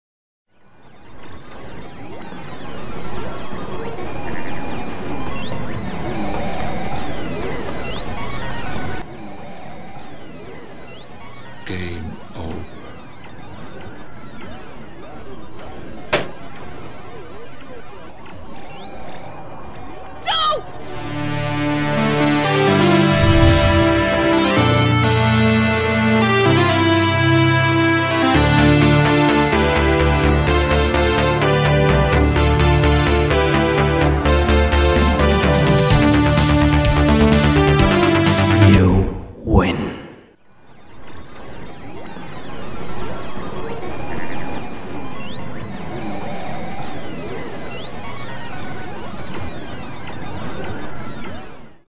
ARCADE ANARCHY - Original Short Film by Hannah Ussery
Hello! My name is Hannah Ussery! Arcade Anarchy is my original Short Film made over the course of 12 weeks! Created for UF Digital Arts and Sciences Senior Project.